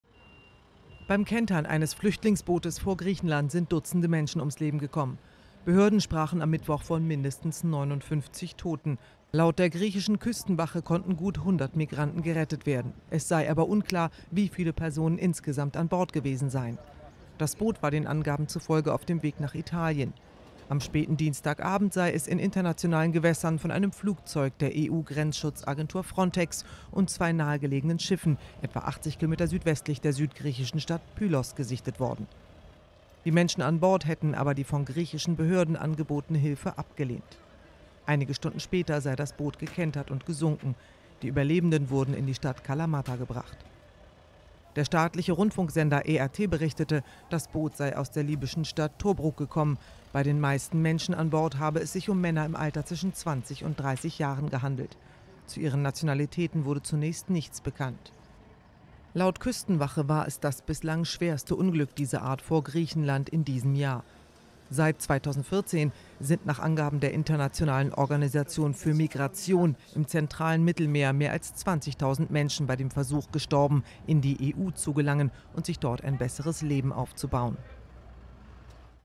Flüchtlingsboot vor Griechenland gesunken: Dutzende Tote
Laut Küstenwache handelt es sich um das schwerste Unglück dieser Art vor Griechenland in diesem Jahr. Rund 100 Menschen konnten gerettet werden. Wie viele sich an Bord des Bootes befanden, ist allerdings unklar.